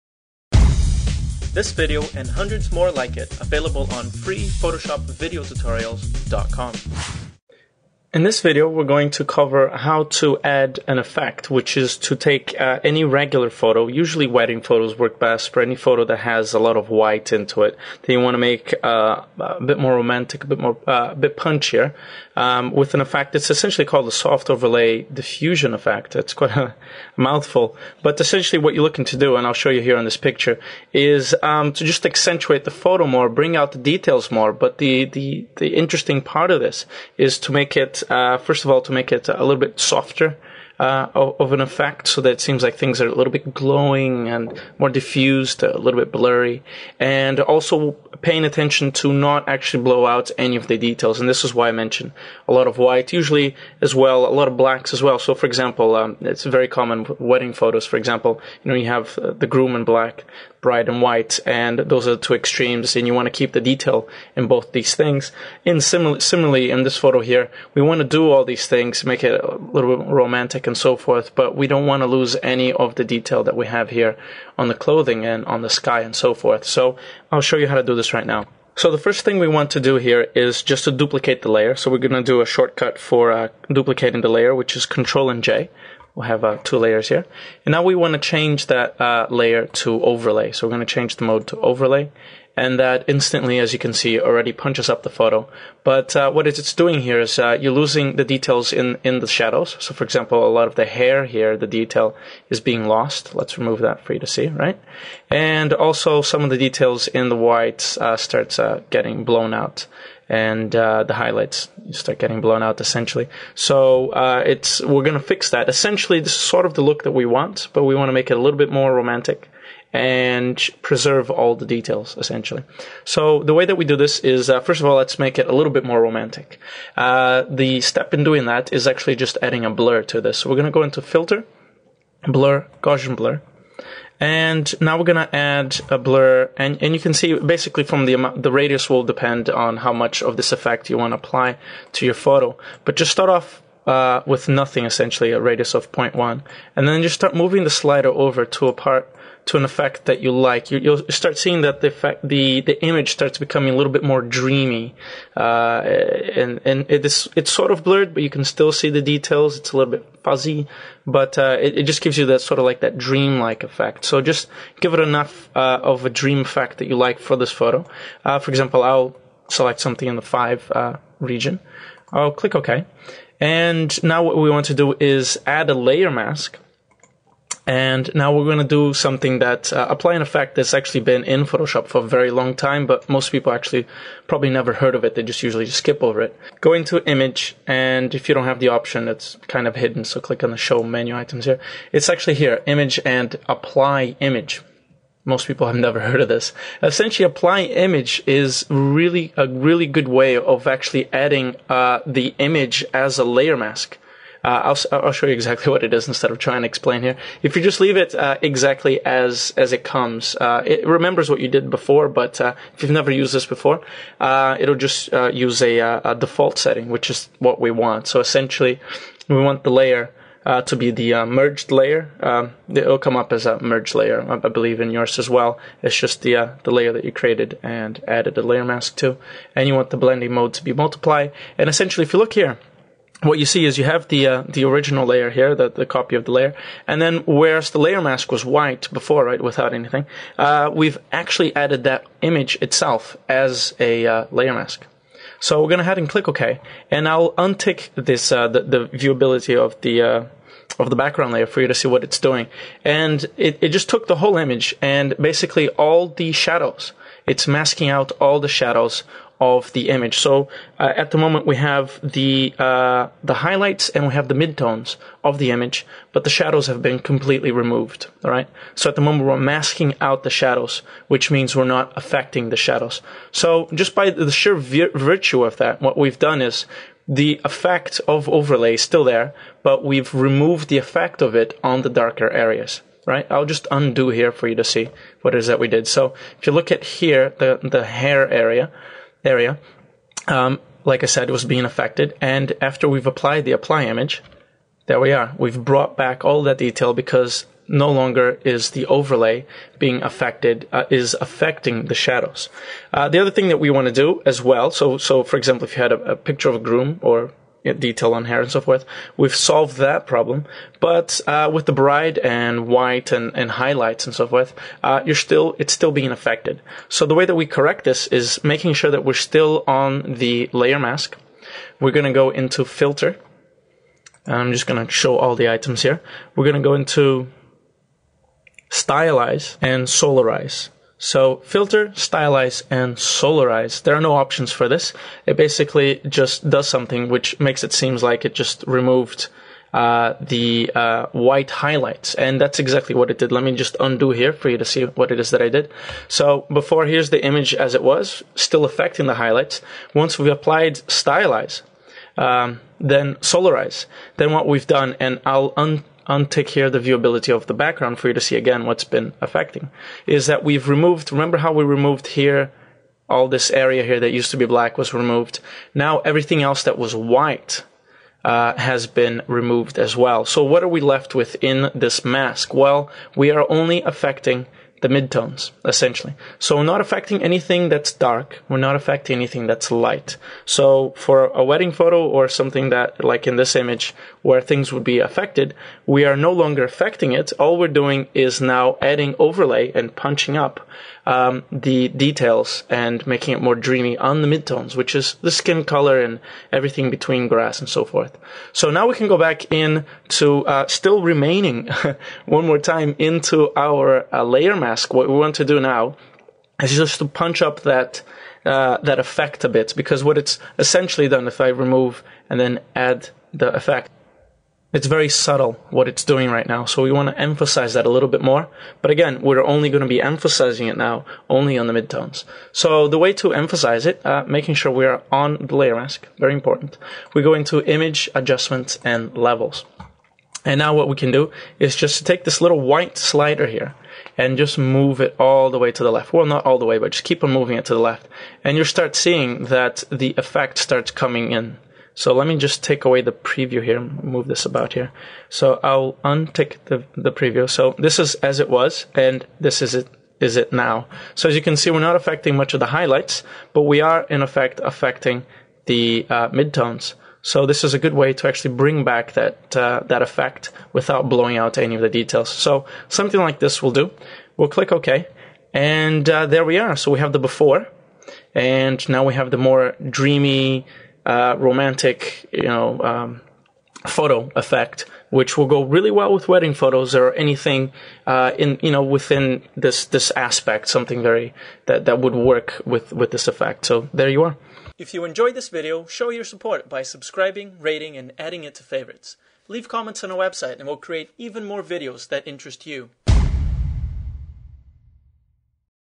Learn Photoshop - How to Add a Romantic Touch to Photos
Learn how to add a romantic, punchier touch to a photo with a soft overlay diffusion effect. Here we cover the use of the overlay blend mode, gaussian blur and solarize filter, use the apply image and levels commands to achieve the effect.